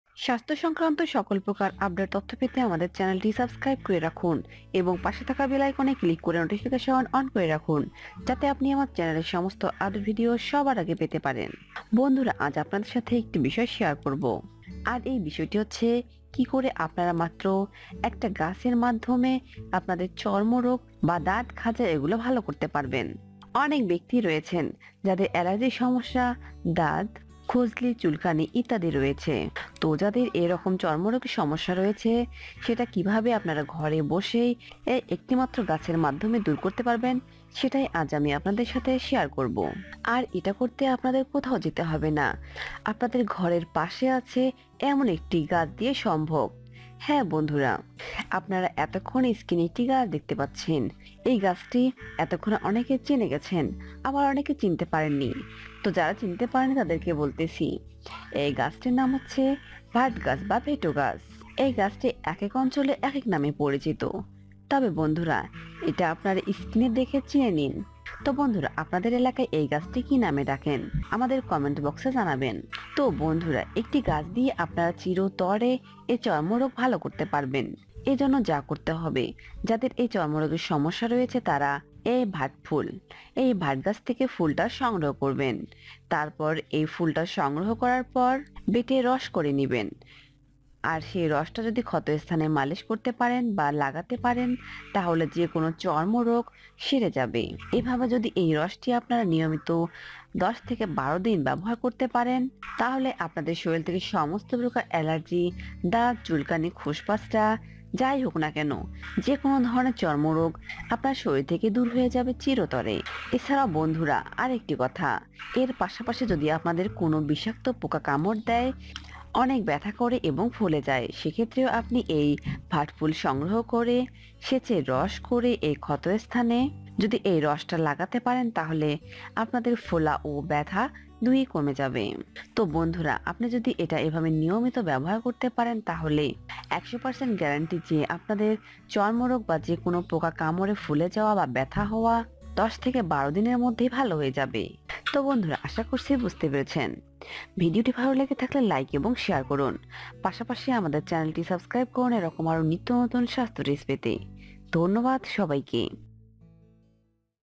৯০ বছরের পুরাতন দাদ, চুলকানিসহ যে কোন চর্মরোগ ভাল হবেই মাত্র ১ বার ব্যবহারে।
৯০ বছরের পুরাতন দাদ, চুলকানিসহ যে কোনচর্ম রোগ ভাল হবেই মাত্র ১ বার ব্যবহারে।

আরও ভিডিও সমুহ =